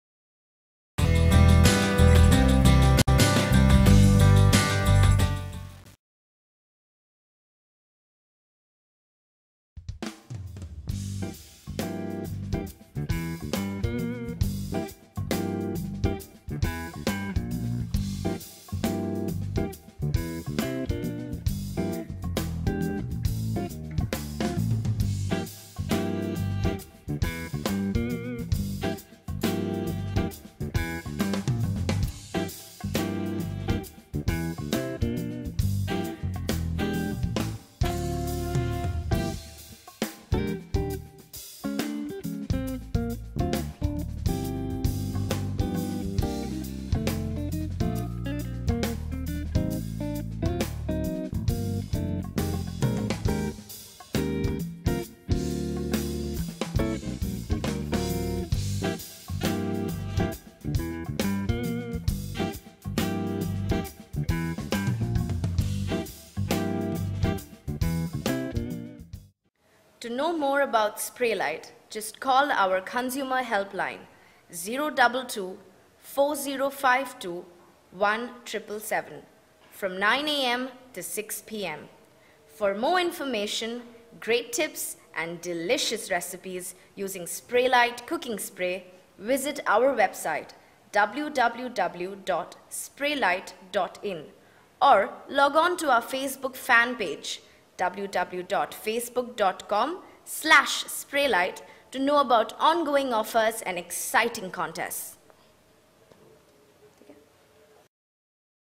Grill Sandwich with Spraylite Cooking Spray
Learn how to make sandwich with Spraylite cooking spray.